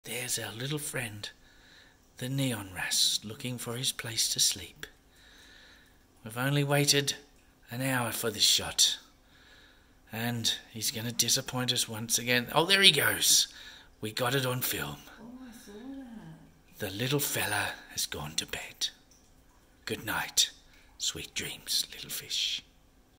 Neon wrasse fish burying itself in Sand
Neon wrasses like to sleep in sand beds.   We spend over an hour trying to capture this one night and finally nailed it.  The attempt at the David Attenborough voice was just to keep myself entertained as it took a long time to get.  This is looking side on down a CADE 1800 fish tank that was only up for about 6 weeks (very young tank).